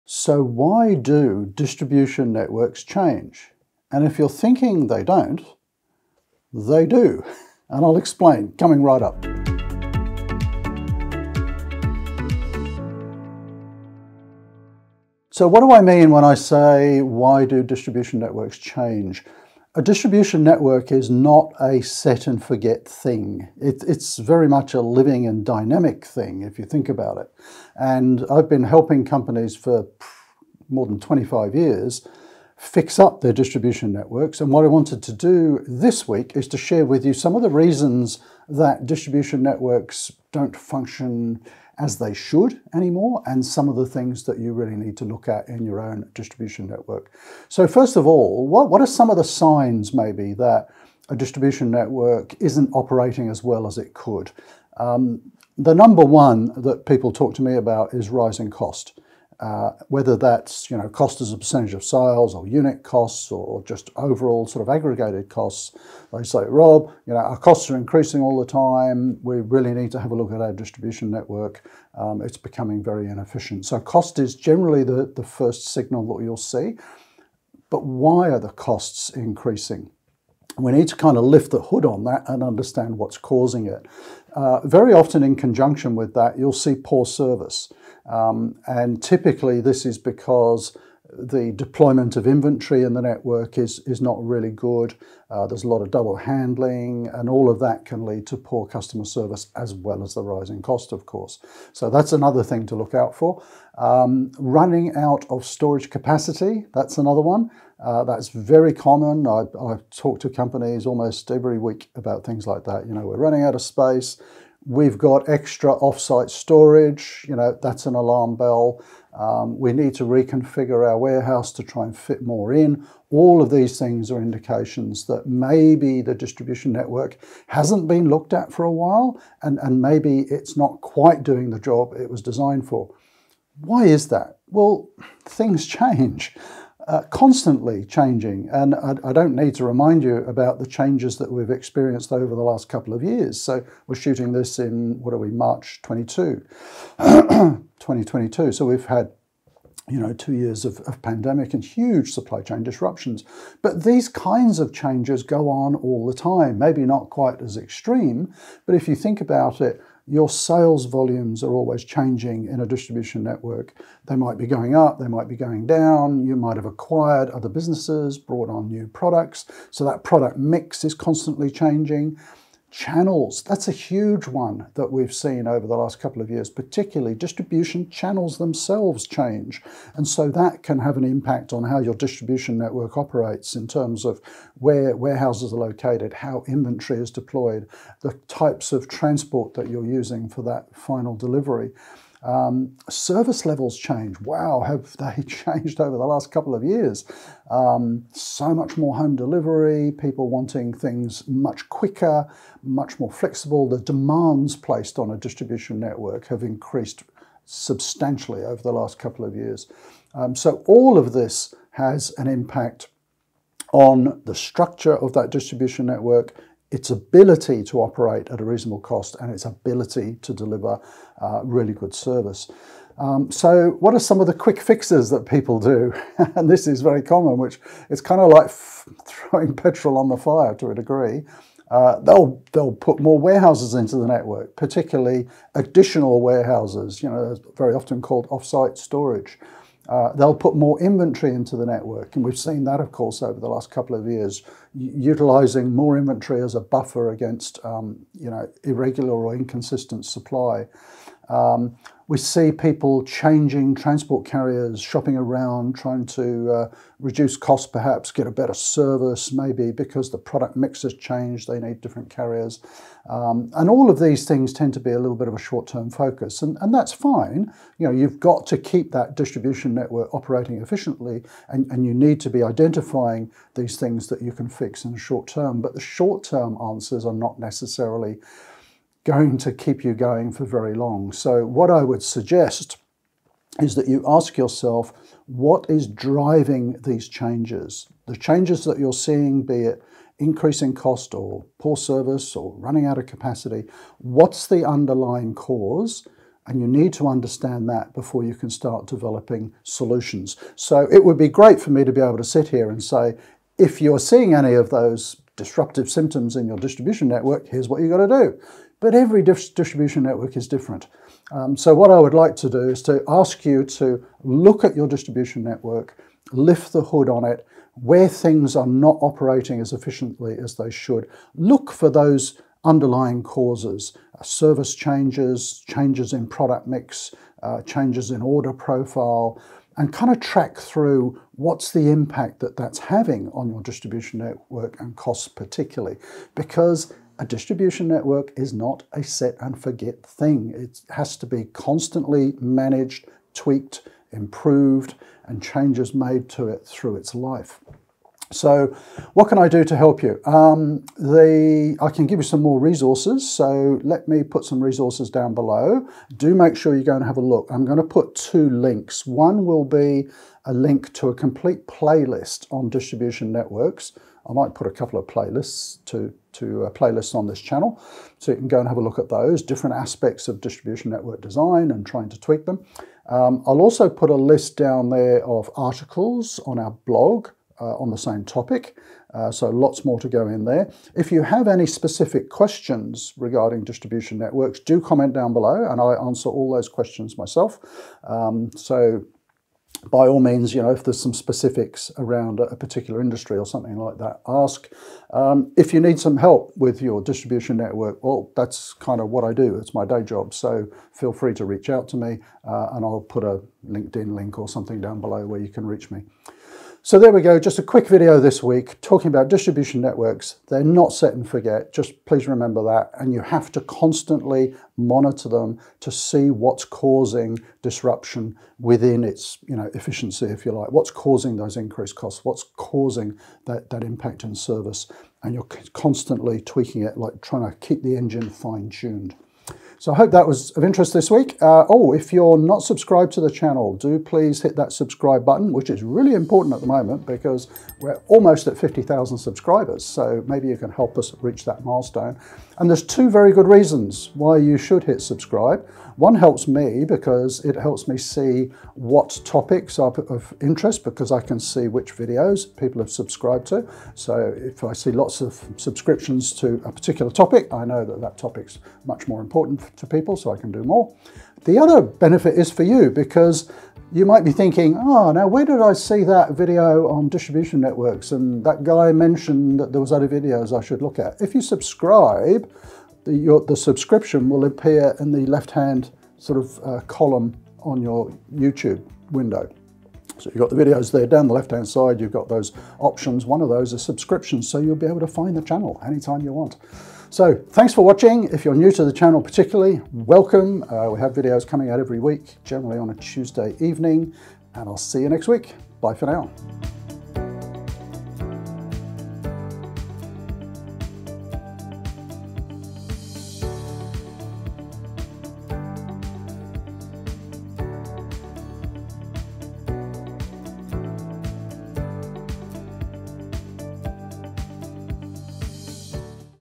The Key Reasons Why Distribution Networks Constantly Evolve
Yes, all Distribution Networks change over time. Or at least they should do.
Here are some of the reasons that the requirements of your Distribution Network might change over the years.  And some things to look out for.

TIMESTAMPS (in case you want to skip) 👇

0:00 Intro
0:47 Cost
1:44 Running out of Storage Capacity
2:52 Channels
3:13 Service Levels

TIPS
3:43 Short-Term Interventions
4:41 Personal Tips

6:46 Summary
7:09 Outro

AND   🙋
Do Please comment with your thoughts, suggestions and any questions

Logistics Bureau is my 'day job'.